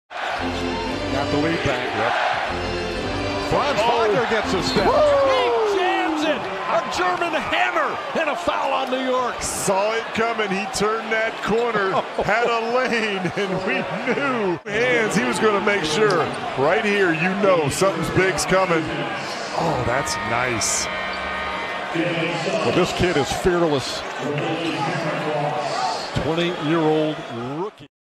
Rookie Franz Wagner | DUNK OF THE YEAR | Magic vs. Knicks | German Hammer
Rookie Franz Wagner | DUNK OF THE YEAR | Magic vs. Knicks | German Hammer | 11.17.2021